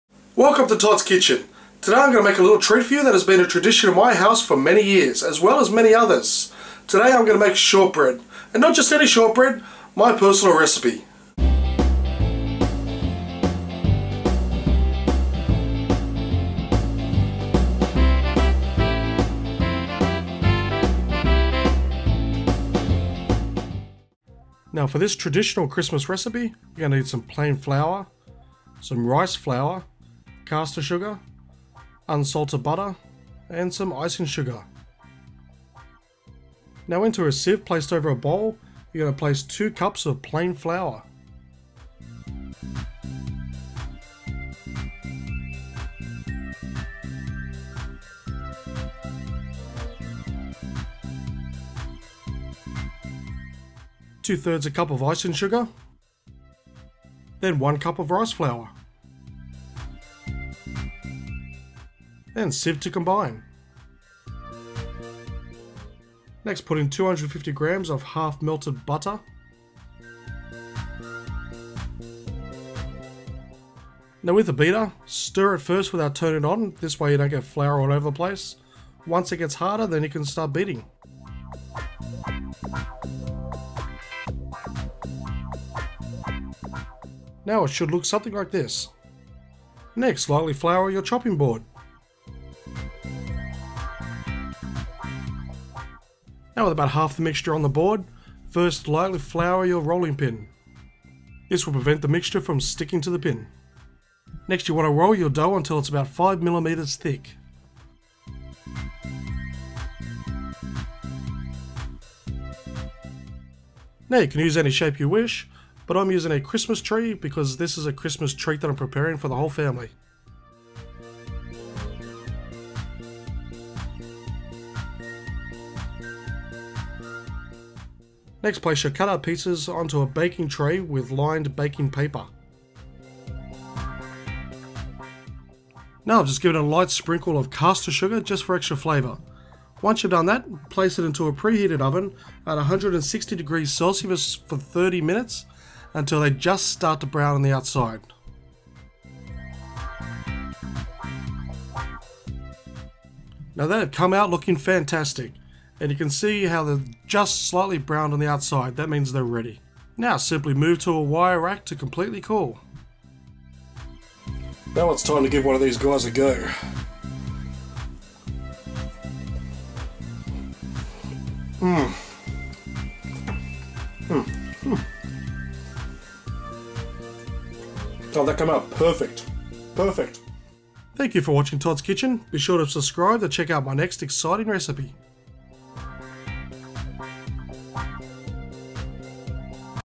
How to make SHORTBREAD
Short bread is very easy to make and can be made for many different occasions.

My Christmas recipe

Ingredients:
250g soft butter
2/3 cup icing sugar
2 cups plain flour
1 cup rice flour
Caster sugar for sprinkling